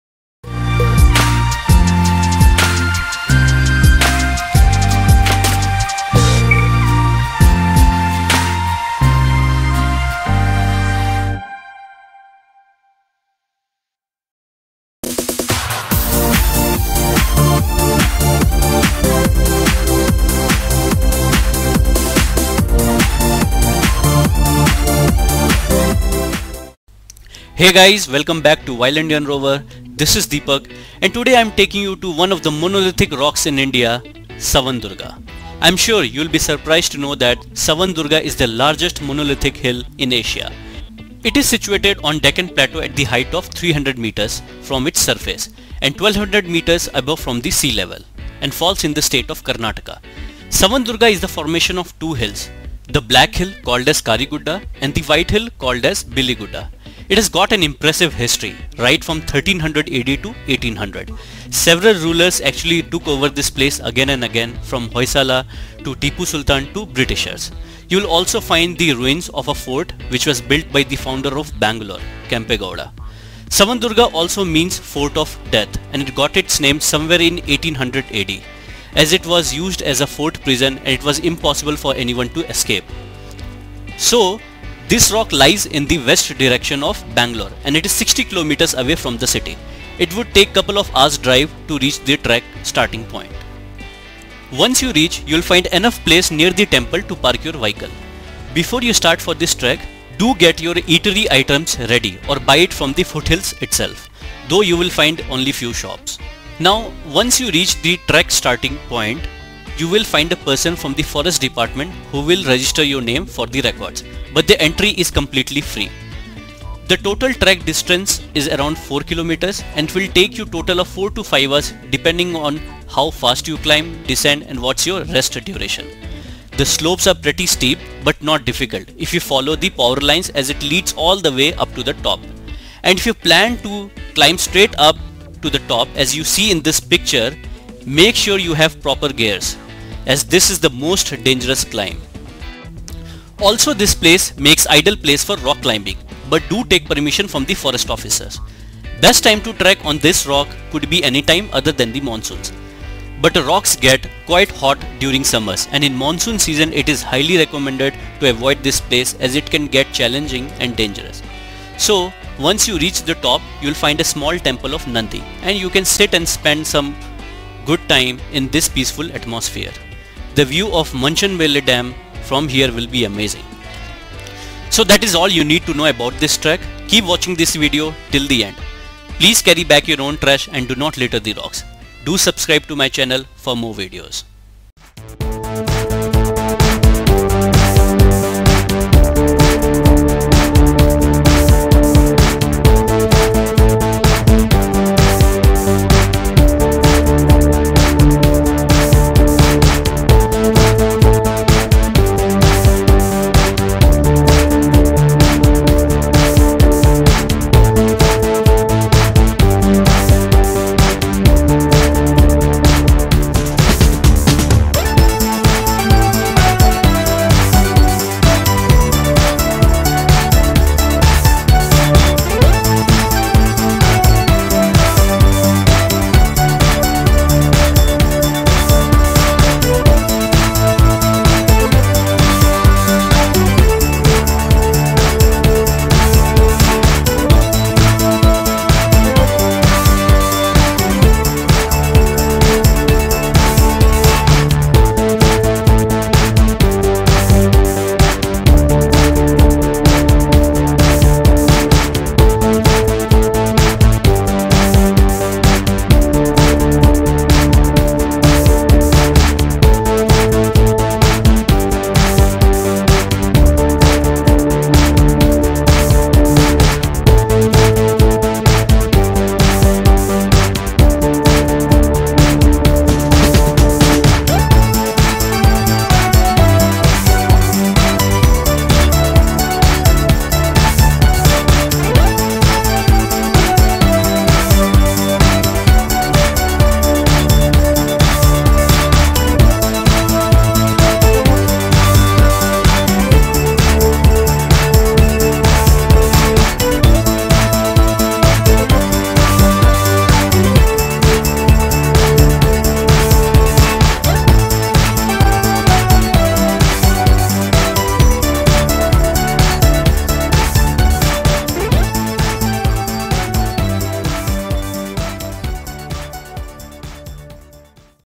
Savandurga | Largest Monolithic Hill in Asia | Savandurga Trek | Bangalore to Savandurga
This video is about largest monolithic hill in Asia called as Savandurga. This rock in situated in deccan plateau and is part of Karnataka. The height of the rock is around 300 m and with an altitude of 1226 m from sea level. It is the formation of two hills, Karigudda(Black hill) and Biligudda(White hill). Several skeletons, copper coins, burial urns have been found at this place. This rock has been captured again and again in the past from 1300 AD to 1800 AD and several rules ruled this place, it is said that the rock was visible from city Bangalore in 1700s. You will also find ruins of the fort which was built by Kempe Gowda II.

Best time to visit : Anytime (But Savandurga Trek not recommended in Rainy season)
Distance from Bangalore : 60 kms
Entry fee : FREE
Trek Distance : 4kms
Trek Duration : 4-5 hrs
Difficulty level : Moderate
Nearby places : Manchanbele Dam

Wild Indian Rover